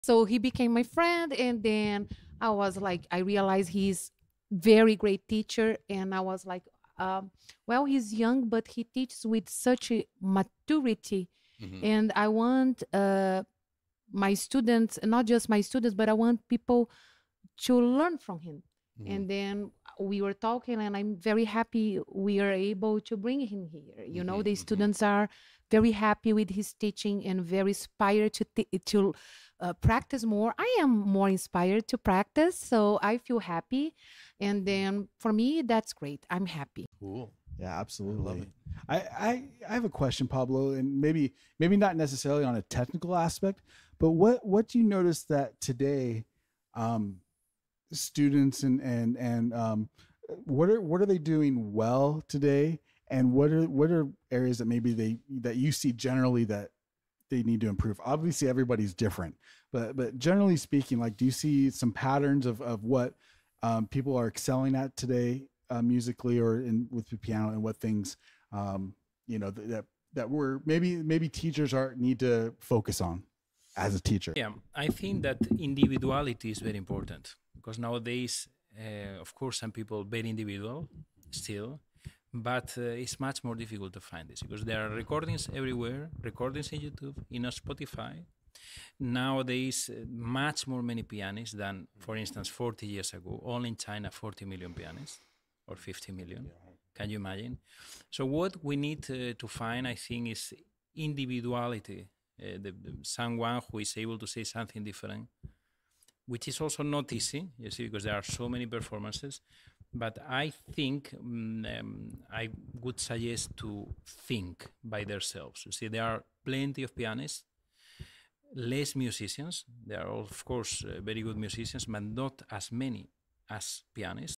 Unlocking Individuality in Music Education and Friendship
The video explores themes of friendship, challenges in music education, and the significance of individuality in the music industry.